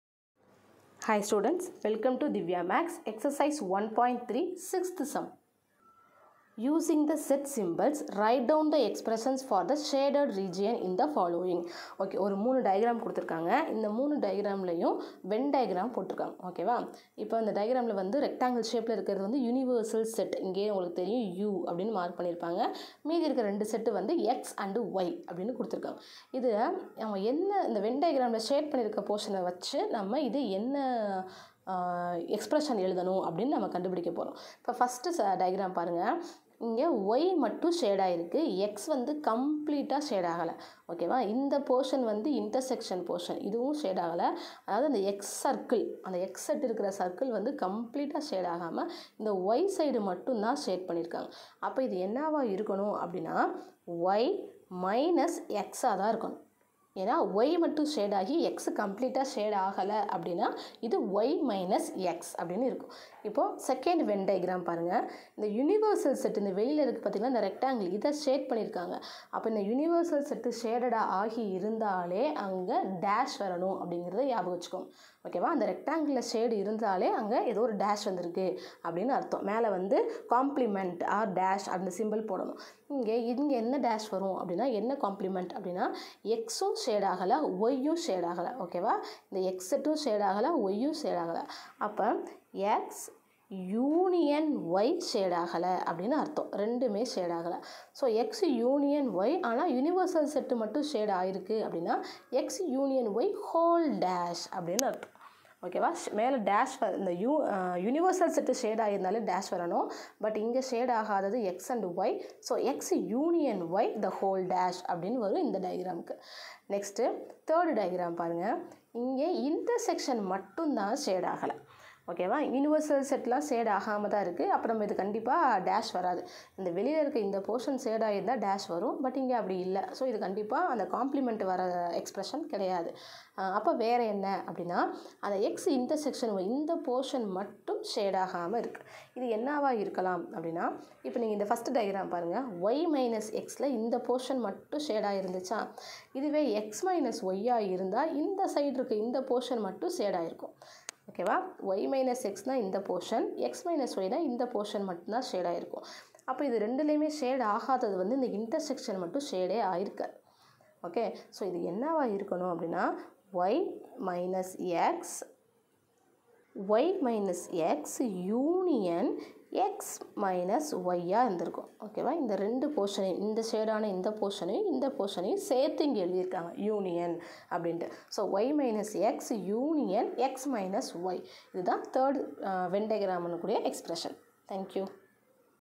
9 th std Maths - Exercise 1.3 (6)
1. Set language - Writing an expressions for given Venn diagrams.